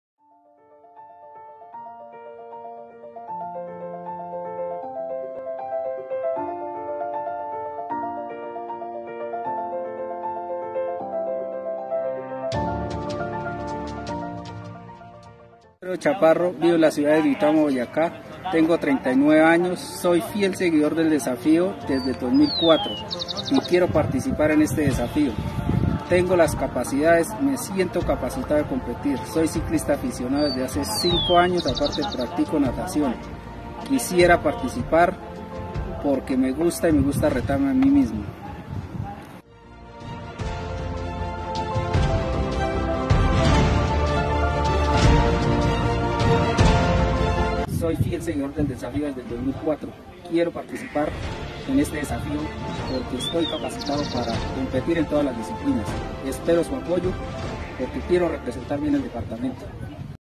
Aspirante al Desafío 2024 del Canal Caracol
Alfred Chaparro hijo de don Pascual Chaparro (habitante de la vereda Quebrada negra municipio de Pajarito) es aspirante al Desafío 2024 en el Canal Caracol;
Deseamos éxitos a Alfred y ojalá todas las cosas le resulten en este propósito representando también al municipio de PajaritoBoyacá